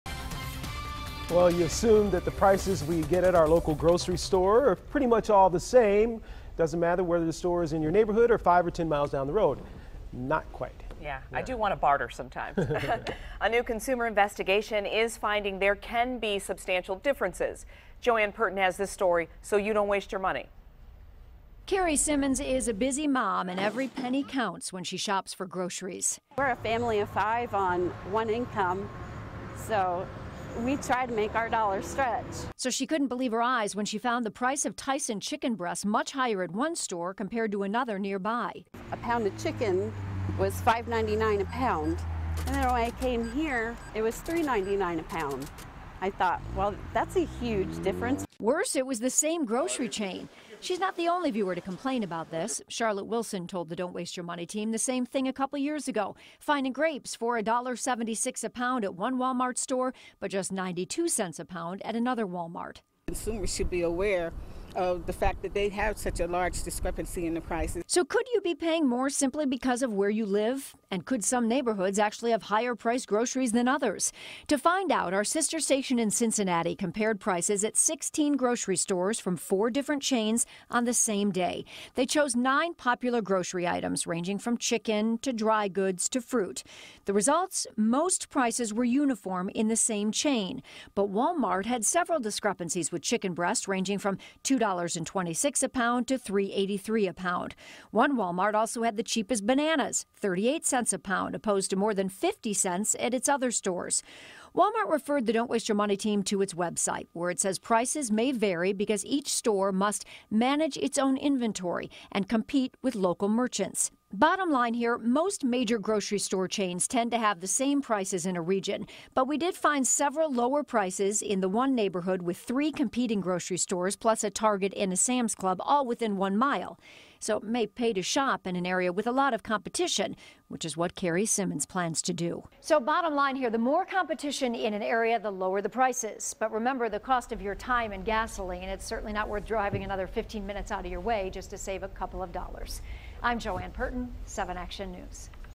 Grocery store prices
We're looking at differences in pricing at grocery stores.